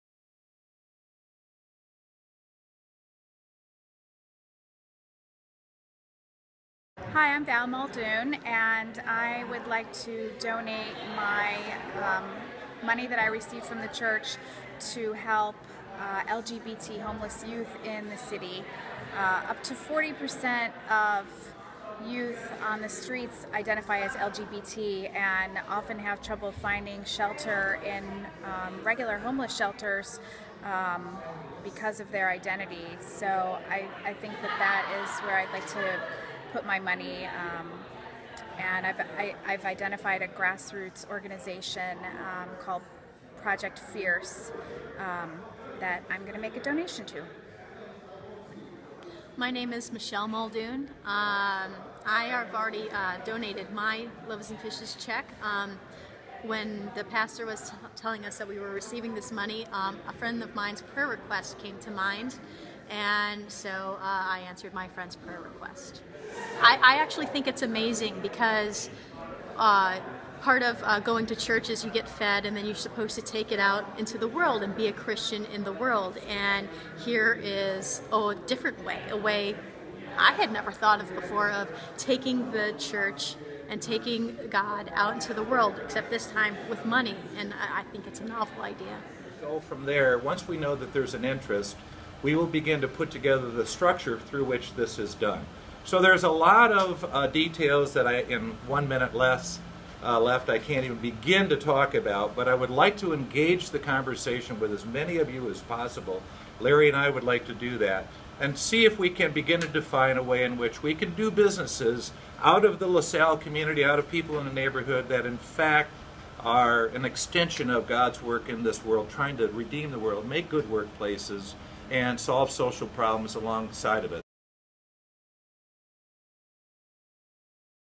LaSalle Street Church
Members of the church shared ideas for use of the money with other members. LaSalle Street Church is sharing part of a $1.6 million windfall from a real estate investment with church congregants who were given $500 each and told to use it the way the lord leads them. | Al Podgorski / Chicago Sun-Times